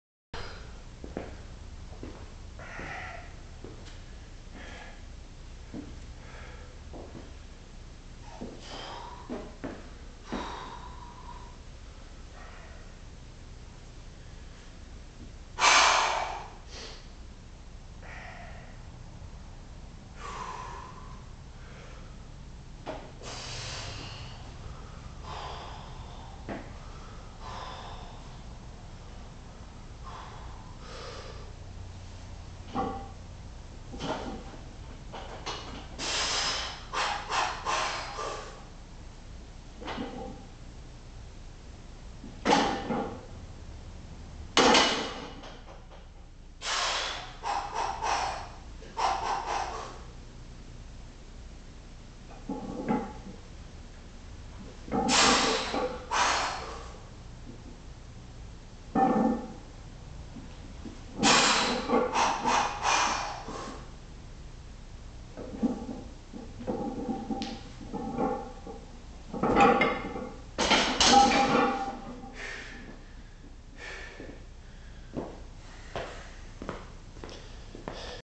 Suit Squat 815x3 @8 5
I finally managed to make some depth in training!  I figured out some technique adjustments and I got there this week despite a tighter suit and knee wraps.  Execution is the key!